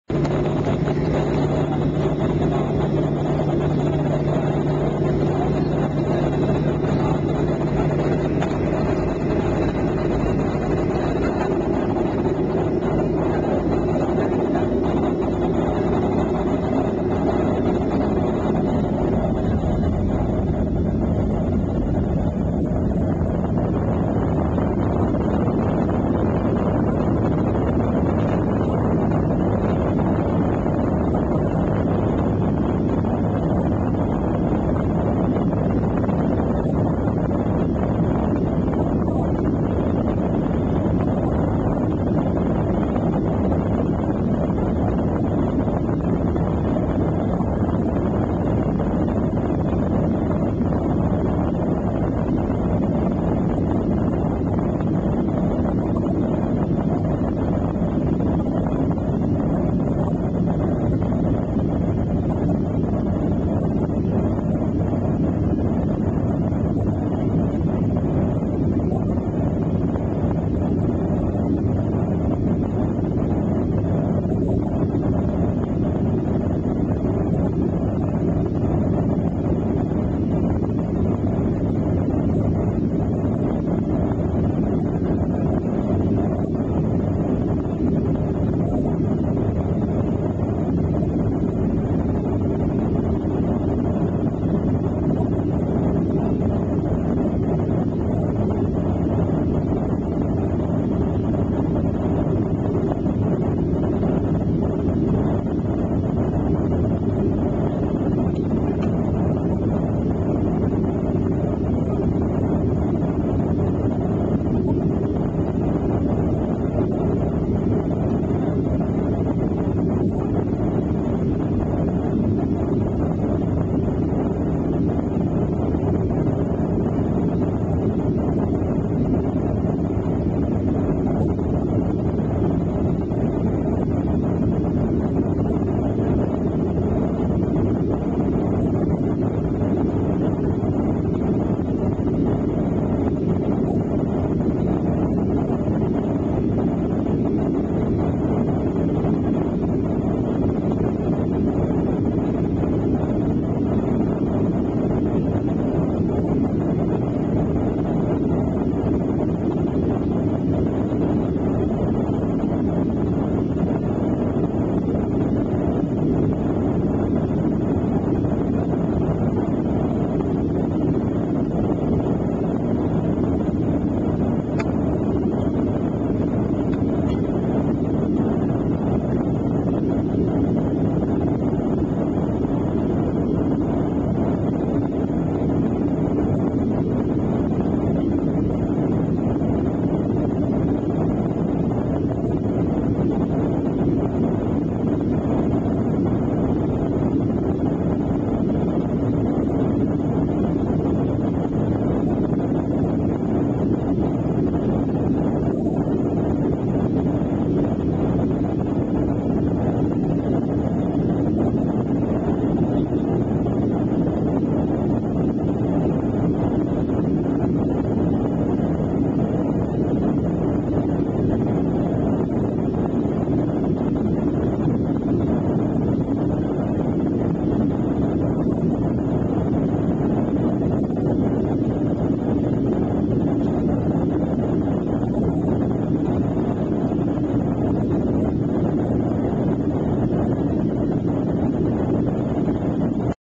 Take-off: From Taxi to the Clouds
The first four minutes of a flight from Tampa to Houston, from the taxiway to above the clouds.